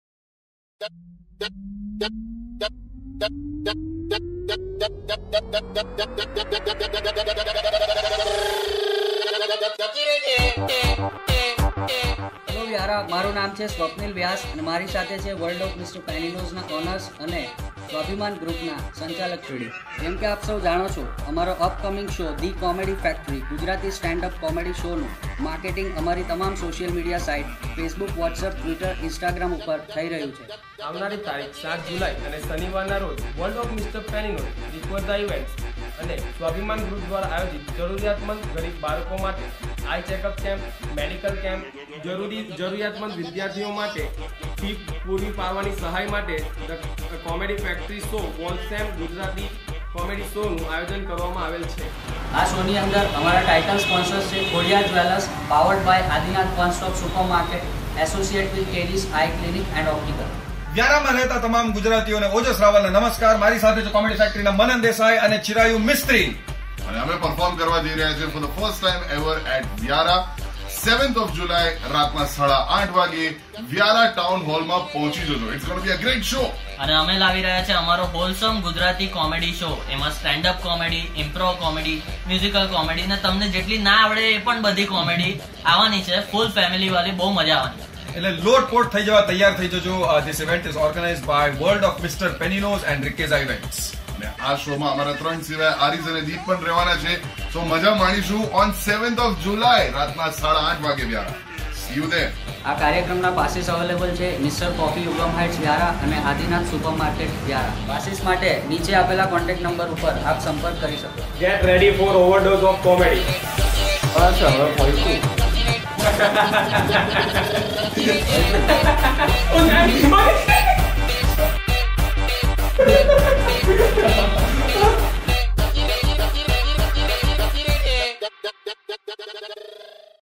Described Details of The Comedy Factory Show organised at Vyara
તારીખ 07 જુલાઈ અને શનિવારના રોજ વર્લ્ડ ઓફ મી.પેનીનોઝ, રીક્વેઝા ઇવેન્ટ્સ અને સ્વાભિમાન ગ્રુપ દ્વાર ગરીબોના લાભાર્થે આઈ ચેકઅપ કેમ્પ, મેડિકલ કેમ્પ, અને વિકલાંગ બાળકોના અભ્યાસ માટે પુરવઠો પૂરો પડવાના ઉમદા હેતુ થી યુવાઓંમાં લોકપ્રિય ગુજરાતી હાસ્યકલાકારો નો લાઇવ શો "The Comedy Factory" સ્ટેન્ડઅપ કોમેડી શો નું ટાઉન હોલ વ્યારા મુકામે સાંજે 08:30 કલાકે આયોજન કરવામાં આવ્યું છે...

ઉલ્લેખનિય છે, કે ધ કોમેડી ફેક્ટરીના કલાકારો

મનન દેસાઈ - ઓજસ રાવલ - ચિરાયુ મિસ્ત્રી - દીપ વૈદ્ય - આરીઝ સૈયેદ તમામ કલાકારો યુવાઓંમાં ખુબજ લોકપ્રિય છે જેઓને યૂટ્યૂબ અને સોશિયલ મીડિયા પર લાખોની સંખ્યામાં લોકો ફોલો કરે છે એમને લાઇવ જોવા માટે વ્યારા તથા આસપાસના વિસ્તારના યુવાઓં આતુરતાથઈ રાહ જોઈ રહ્યા છે...

શો ના પાસ માટે પ્રોગ્રામ ના આયોજકો

સ્વપ્નીલ વ્યાસ, વૈભવી શાહ પટેલ, ચિરાગ જોષી અને મિતુલસિંહ સૂરમાં નો સમ્પર્ક કરવો જરૂરી છે...